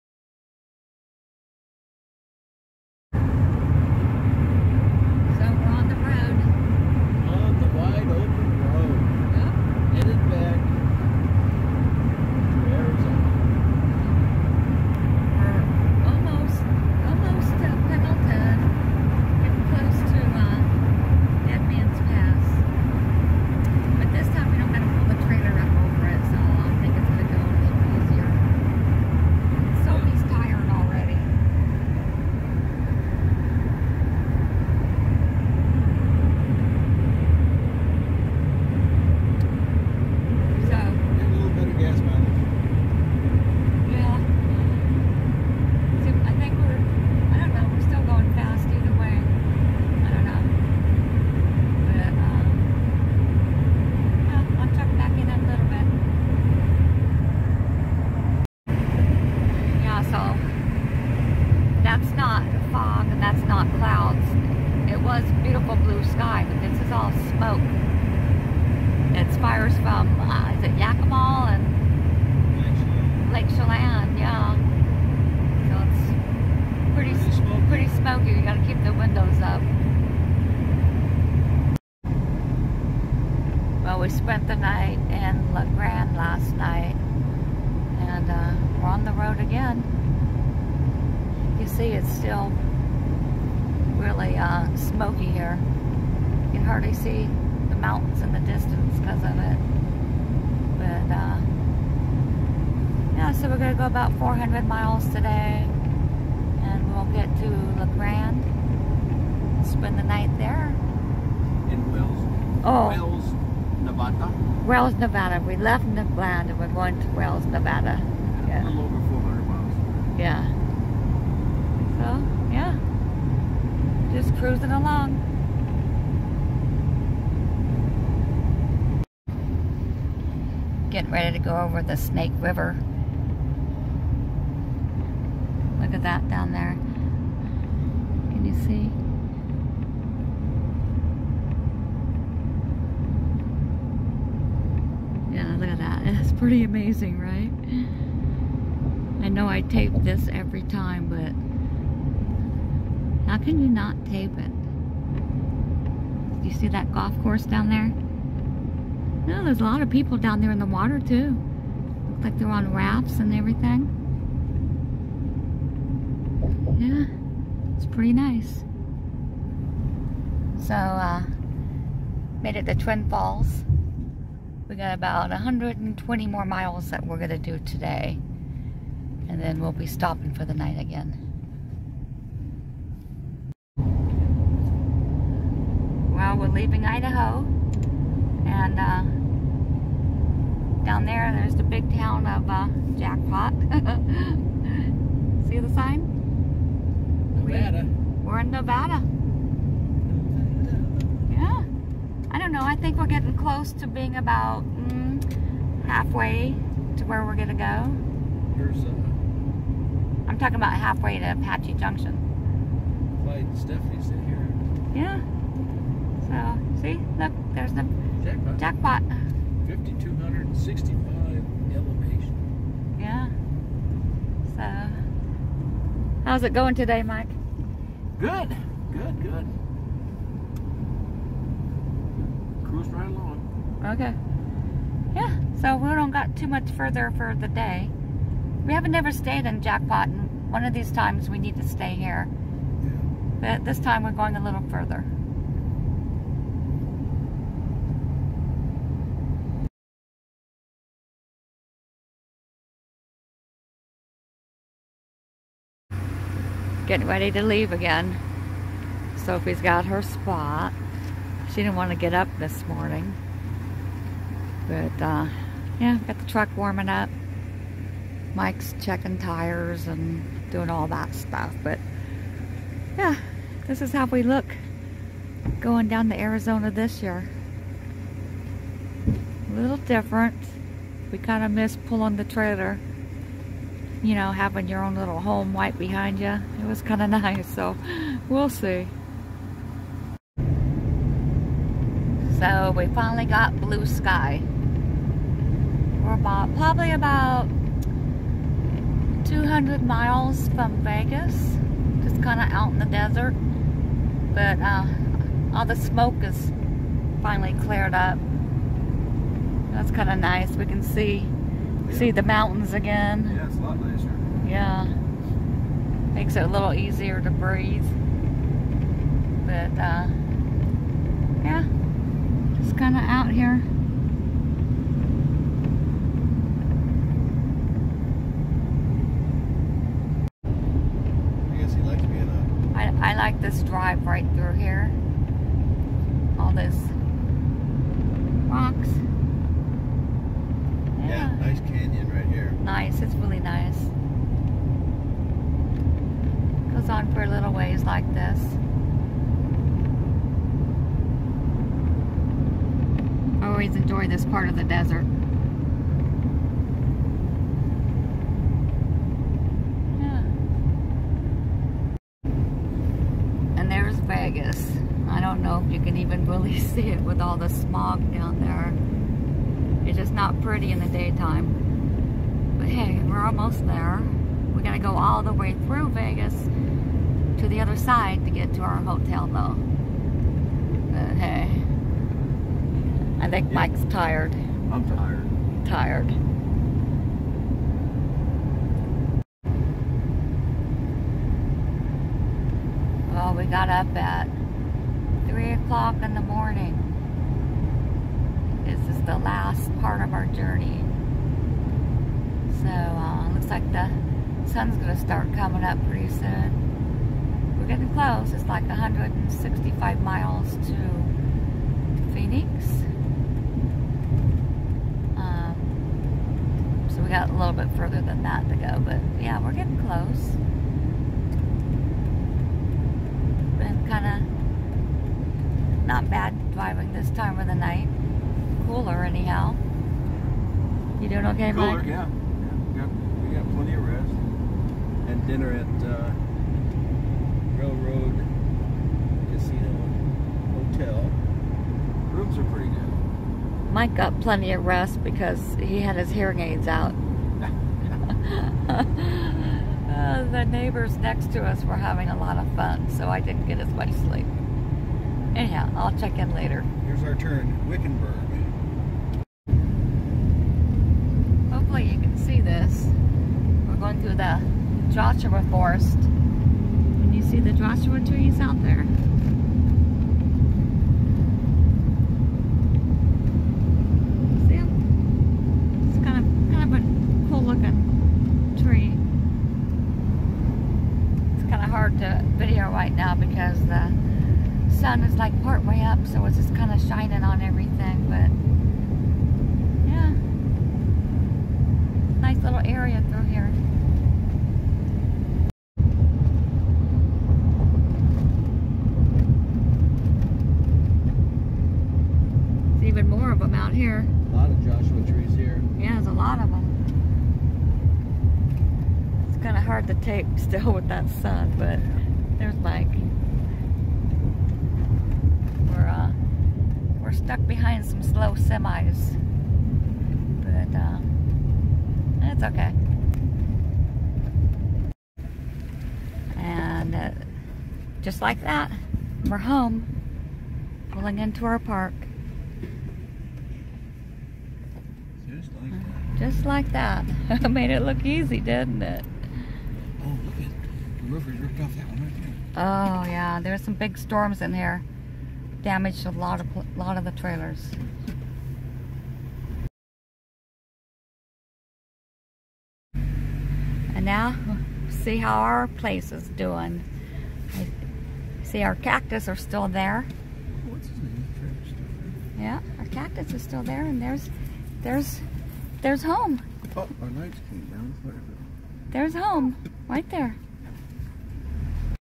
We leave Washington and Arrive in Arizona to our winter home, Towing the Prius
We left Washington earlier than we had planned and its still very hot here. We had a nice drive to Arizona. This time pulling the Prius instead of our trailer. Were not sure which one we prefer.. it was nice having our home travel with us.